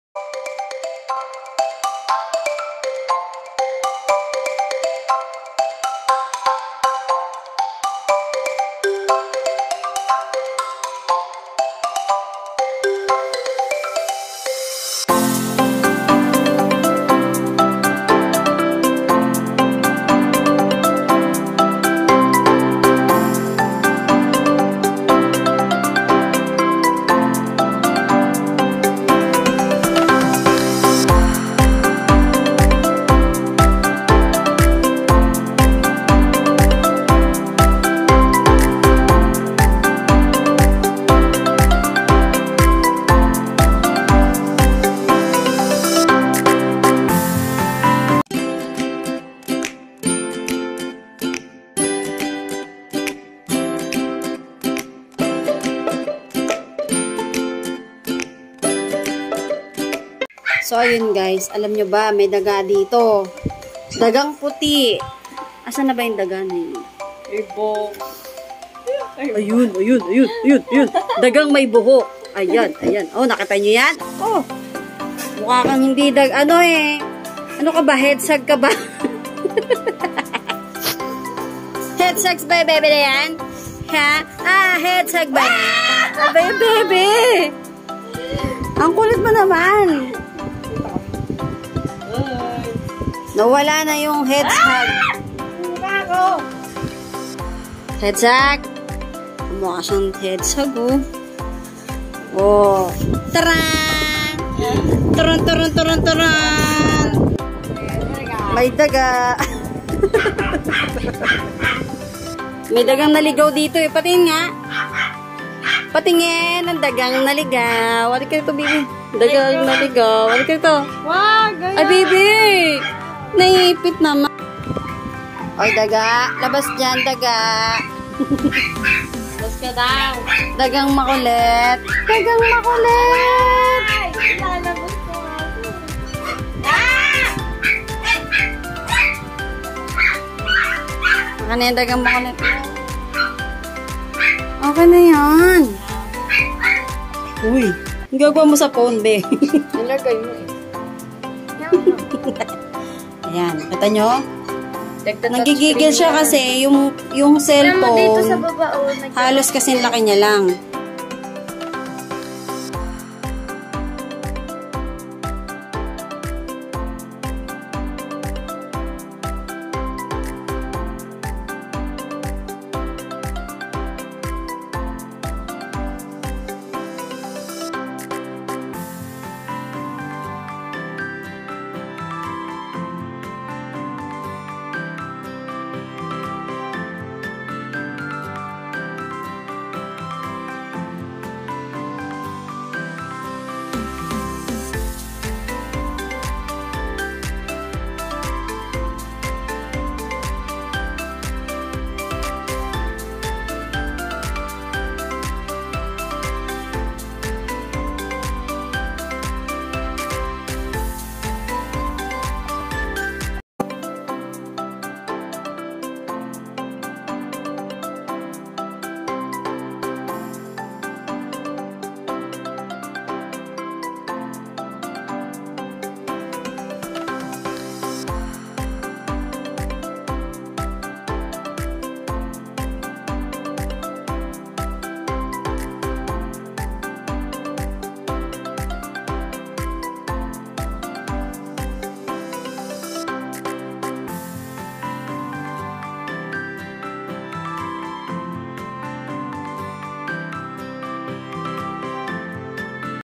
Pomeranian Collectible Size |Sobrang liit mo🥰
Meet my Collectible size pomeranian puppy na sobrang cute